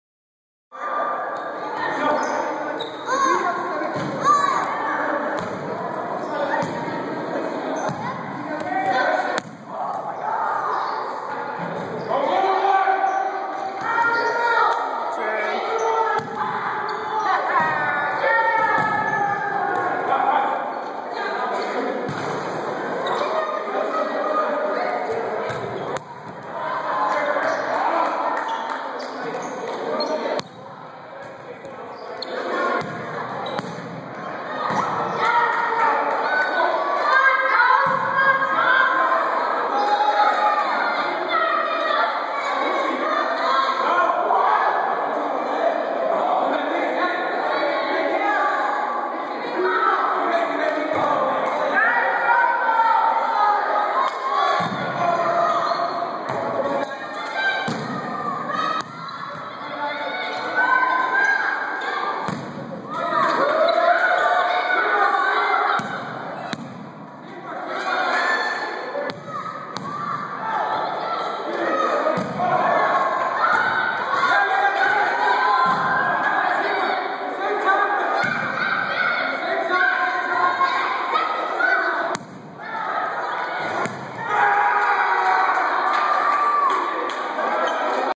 Epic Dodgeball Standoff Doug Vs. Rich
Dodgeball Tournament at Stroudsburg Wesleyan Church